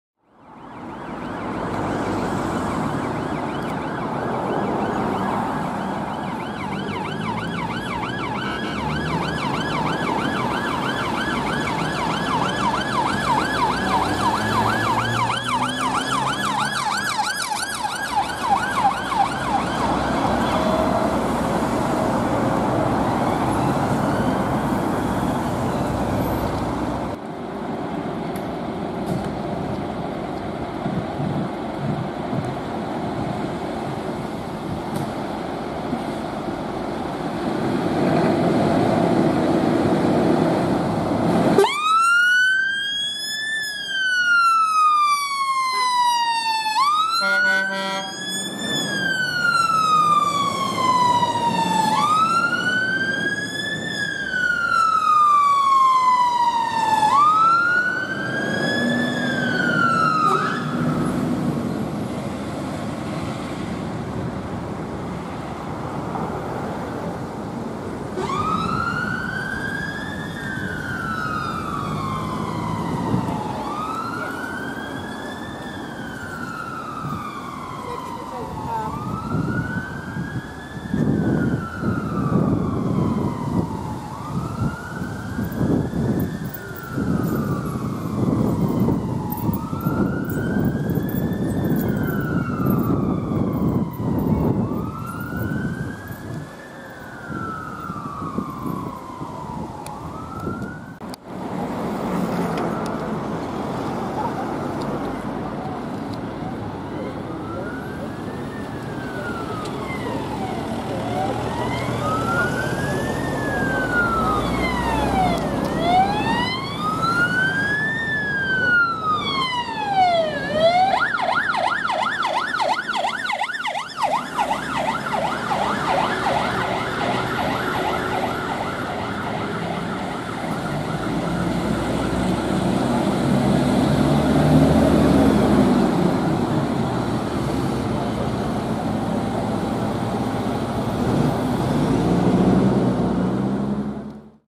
*AIRHORN!* Cambridge Pump Turning Out on Opposite Lanes! + Police and Ambulance Responding!
Cambridge's Second Pump C01P5 turning out to a fire in the open in Sawston, Cambridgeshire going opposite lanes due to traffic!

0:00 West Midlands Police Peugeot 308 IRV responding through heavy traffic on Bristol Road
0:26 Cambs FRS Scania P280 Rescue Pump C01P4 responding to a Fire in the Open in Sawston
1:40 EEAST Fiat Ducato DCA Responding past Cambridge Fire Station

My Twitter a Fire in the Open in Sawston
1:40 EEAST Fiat Ducato DCA responding past Cambridge Fire Station